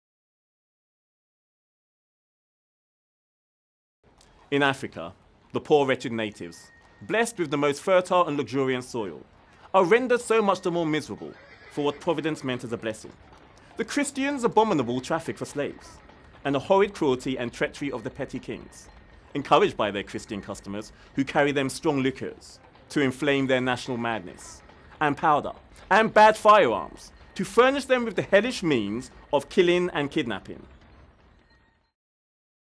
Ignatius Sancho
George Lamptey as Ignatius Sancho, born in 1729 on a slave ship bound for England he became a prominent composer, actor and writer. This is an extract from the Letters of Ignatius Sancho published after he died in 1780. He writes to Jack Wingrave, the son of his friend John Wingrave, the London bookseller and bookbinder